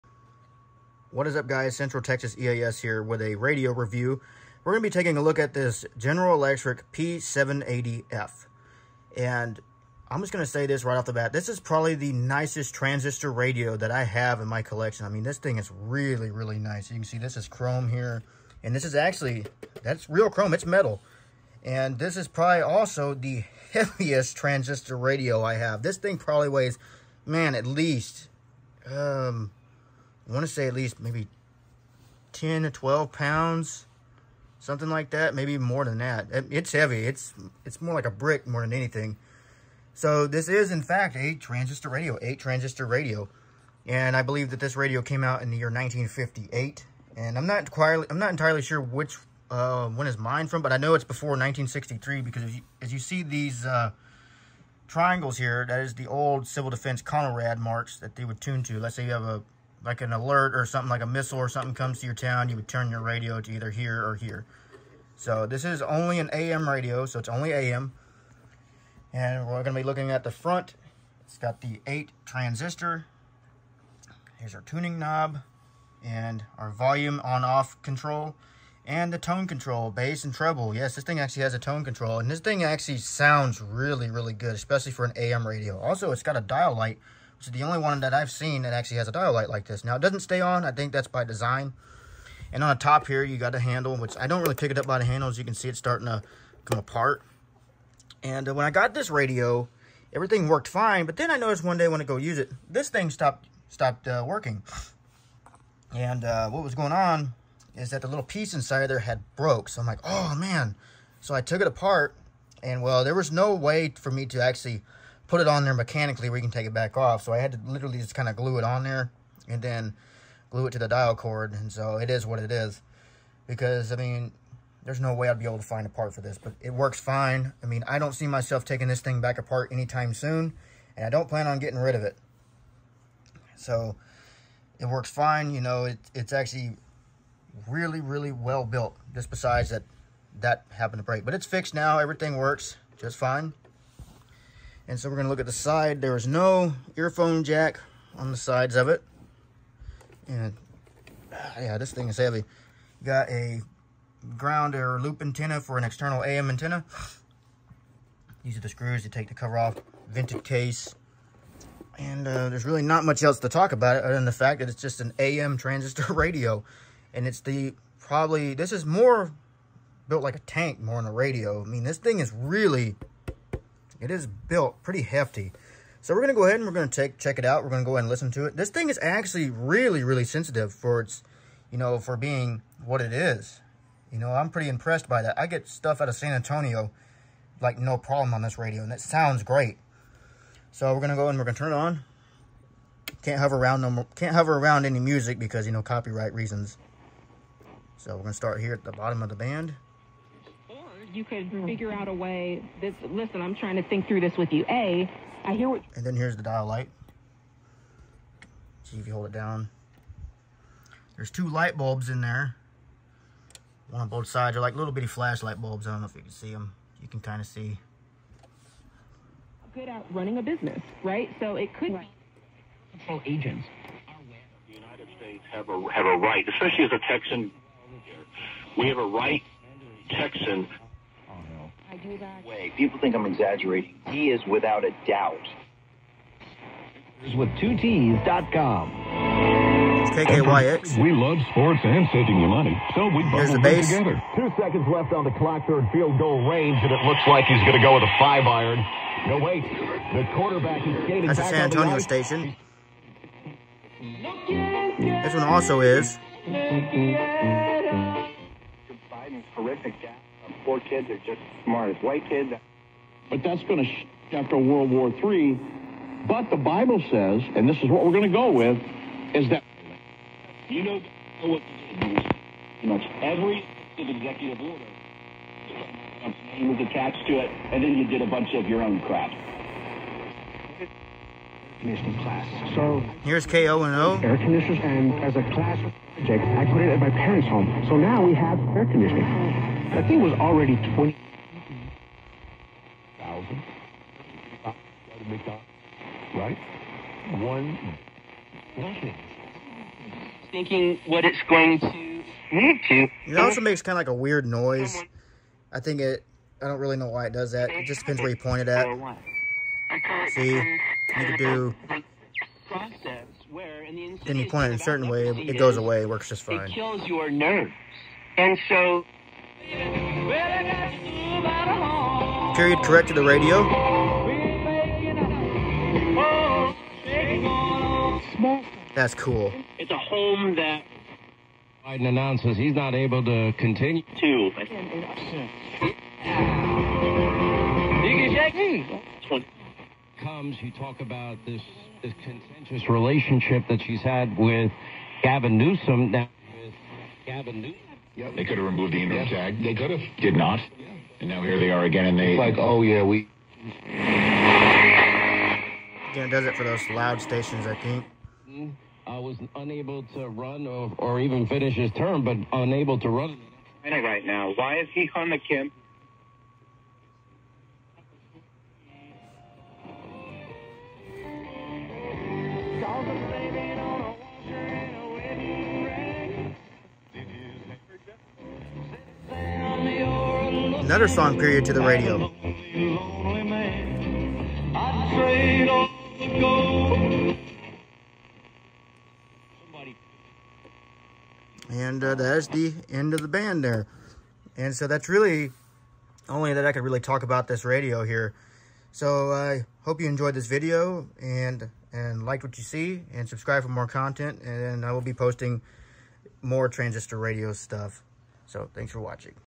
Vintage GE P-780f transistor AM radio review
This is by far the nicest and best sounding transistor radio I have in my collection and still works great ￼ even being over 60 years old. ￼

Date of recording: January-26-2024